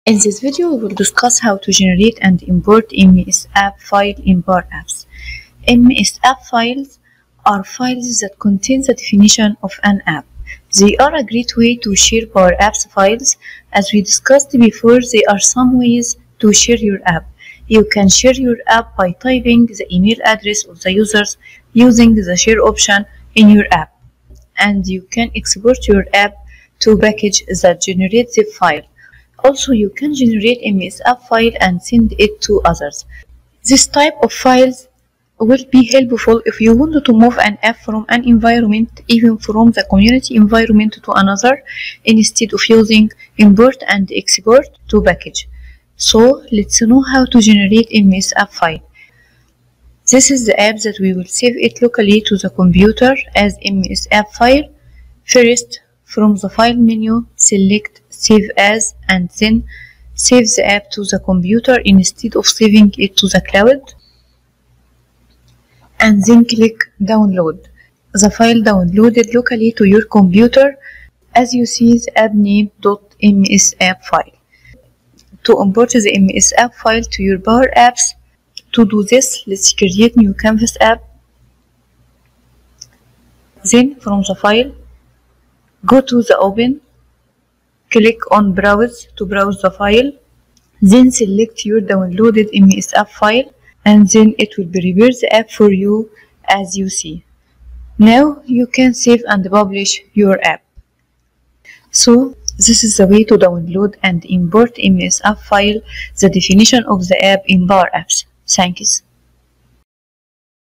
PowerApps generate and import msapp file (English)
MSAPP files, , are files that contain the definition of an app.
 MSAPP files are a great way to share Power Apps files
00:00 What's .MSAPP files in Power Apps
00:17 Ways to share App files
00:50 Generete .MSAPP file in Power Apps
01:15 import .MSAPP  file in Power Apps